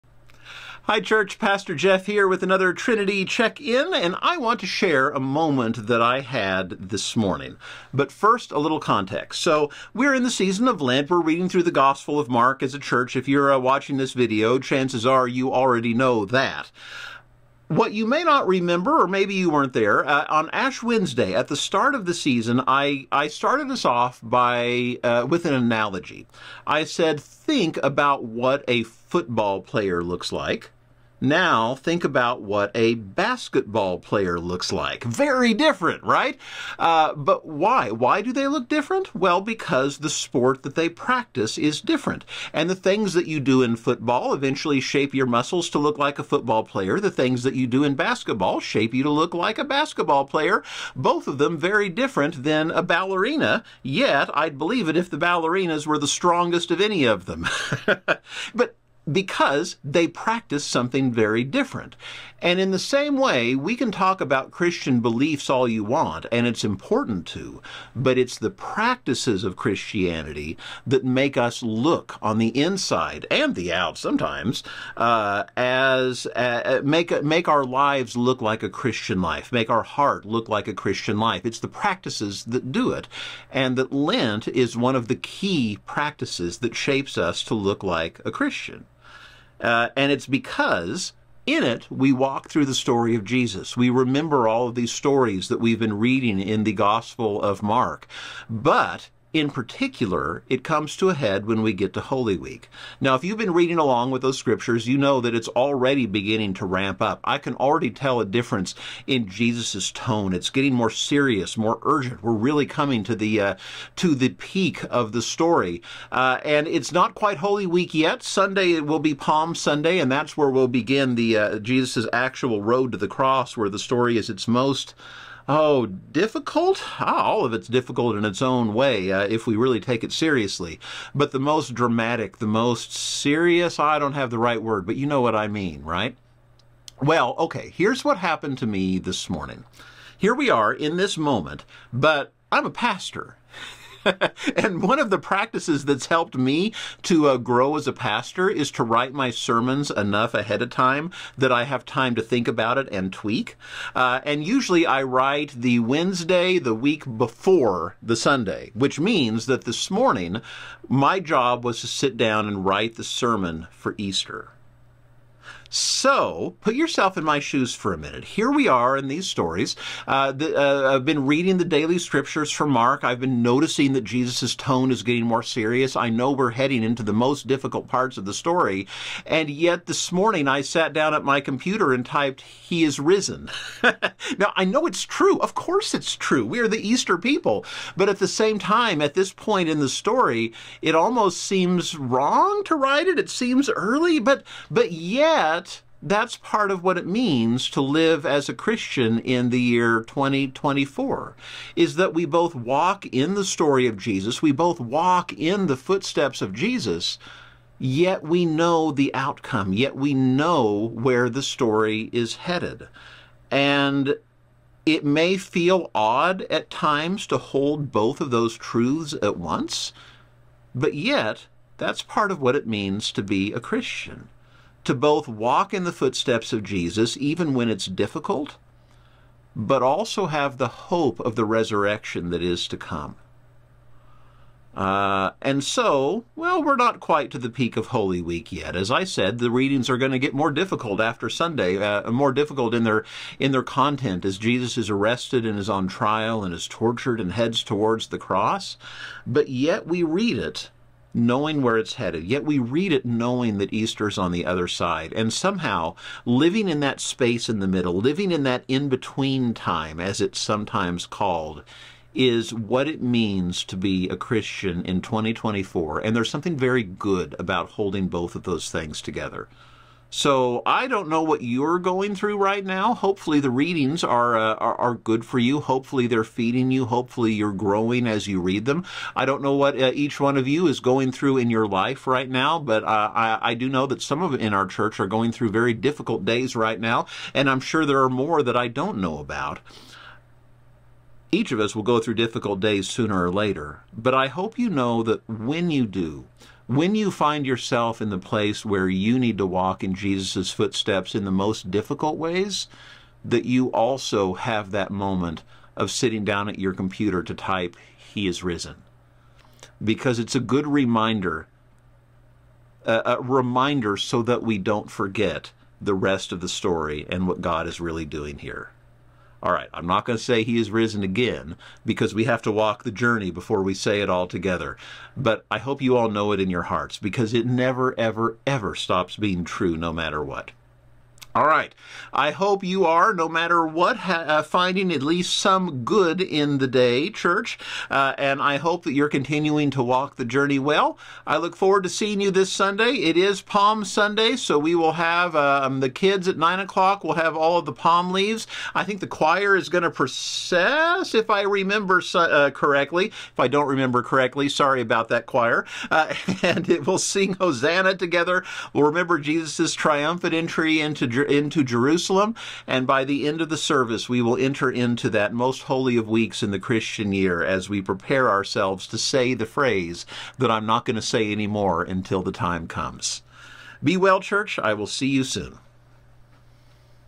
Trinity Check In for March 20, 2024
Join Pastor Jeff as he shares a story of a moment he had this morning, thinking about what it means to be a Christian in this time and place.